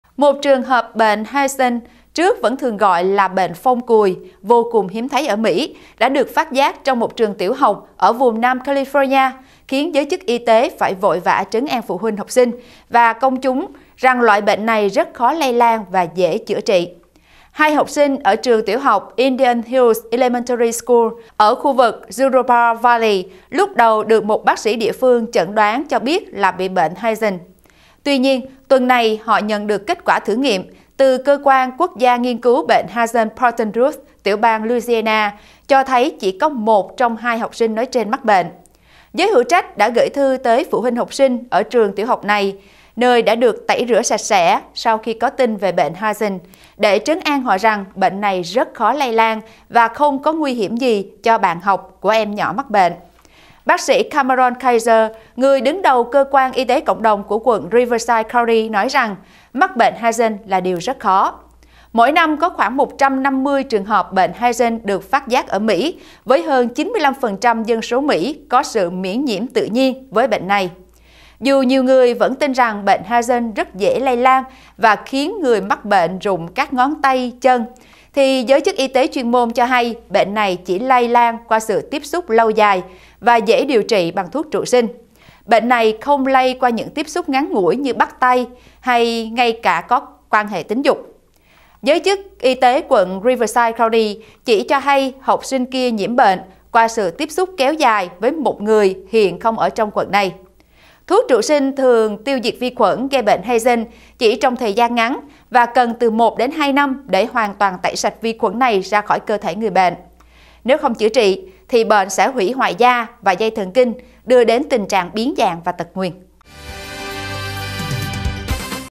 Tìm thấy trường hợp bệnh Hansen’s trong trường tiểu học California
Một trường hợp bệnh Hansen, trước vẫn thường được gọi là bệnh phong cùi, vô cùng hiếm thấy ở Mỹ, đã được phát giác trong một trường tiểu học ở vùng Nam California, khiến giới chức y tế phải vội vã trấn an phụ huynh học sinh và công chúng rằng loại bệnh này rất khó lây lan và dễ chữa trị.